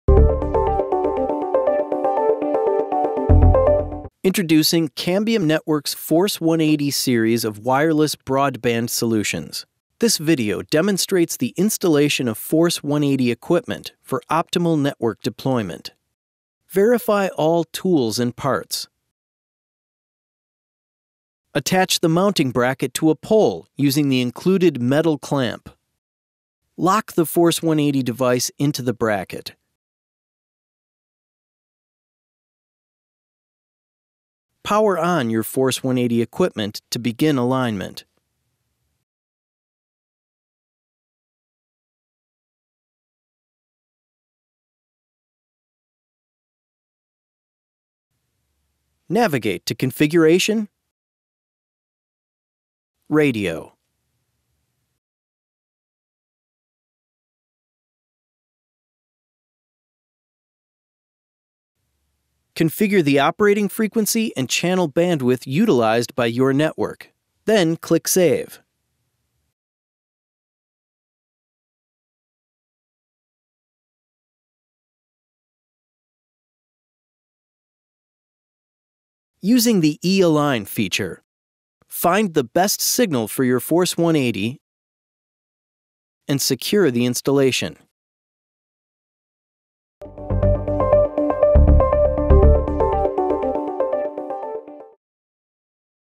How To Assemble ePMP Force 180
Learn how to assemble ePMP Force 180 and get up and running quickly with high quality affordable connectivity.

ePMP Force 180 is designed for providing wireless broadband in high interference outdoor environments, providing up to 200 Mbps of bandwidth. Easy installation with the provided mounting bracket makes getting started easier than ever.